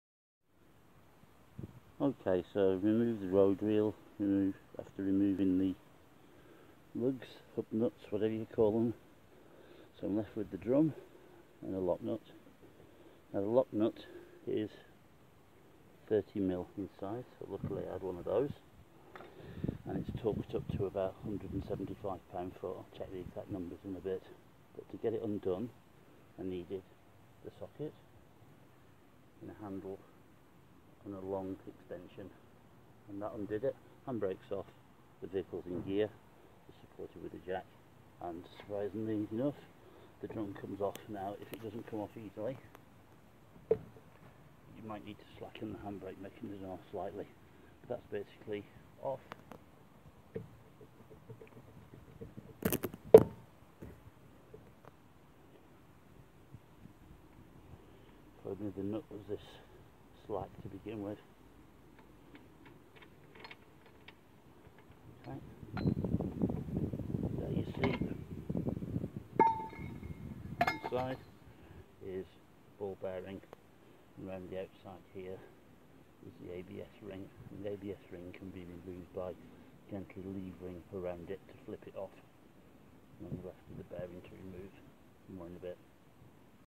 Nissan Note 2006 rear wheel bearing swap 1 of 5
I have put a copy of the RAX section of the Nissan Note Service manual

. DELIGHT in the dimensions of the service tools, check they are right then make your own if you really must!

Removal of the lock nut on the stub axle and extraction of the brake drum containing the wheel bearing and the ABS ring.

The dust cover levers off with a screw driver and it is thrown away as it is replaced with a new one in most bearing kits

You will need a long extension handle and ideally a shallow socket, the nut is tight to 130lb/ft for maybe TEN YEARS so it might not go without a lot more pressure. I used a high quality extension and a length of 38mm steel tube.

If it does slacken rapidly wear hand protection as you might be dropped down quickly.

THe drum should pull away easily assuming you have set the vehicle in gear (or PARK) for safety AND disengaged the parking/hand brake.

If the drum will not pull away easily the handbrake cable may be too tight or the drums badly worn out inside.

IMPORTANT: do not apply the footbrake with no drums fitted the brake pistons could over extend leading to them being  ejected or air entering the system.